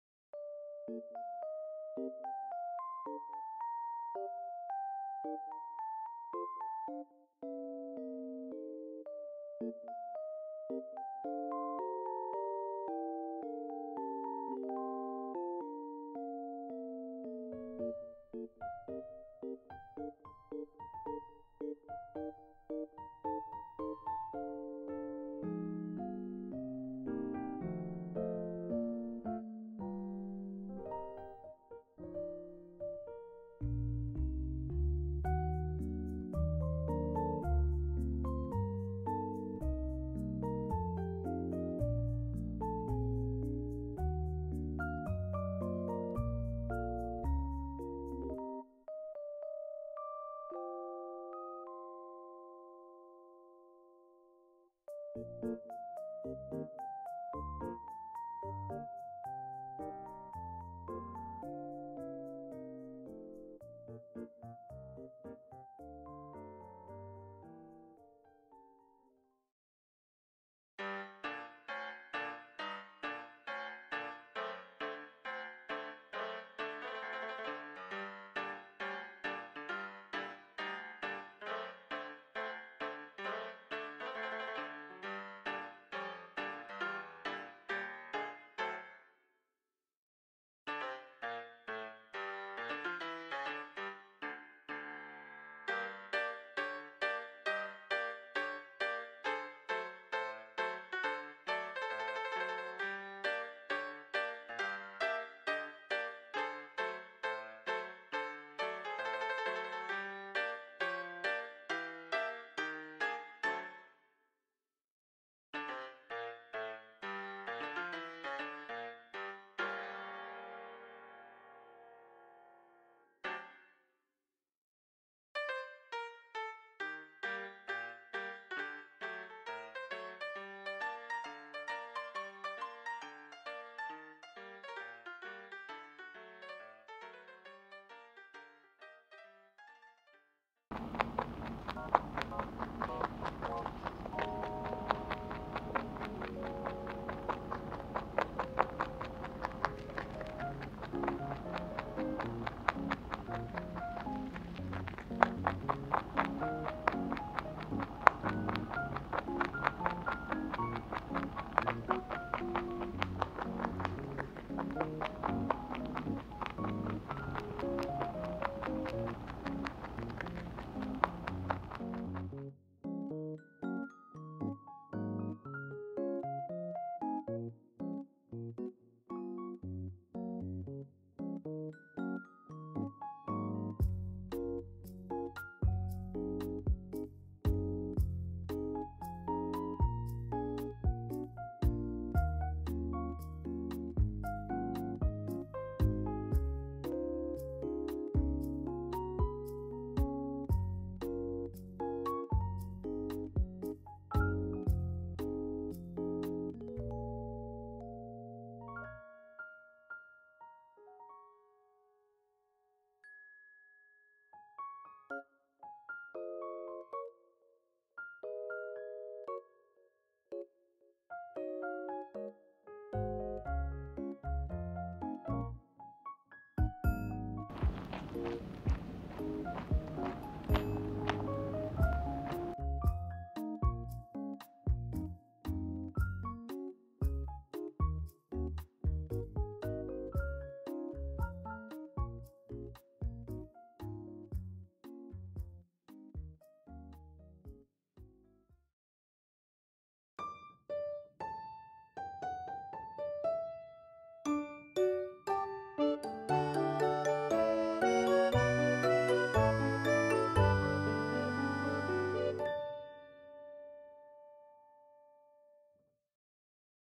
早朝5時からお腹を空かせたウサギが目の前で倒れてました【No.639】
ネザーランドドワーフの麦（むぎ）

使用BGM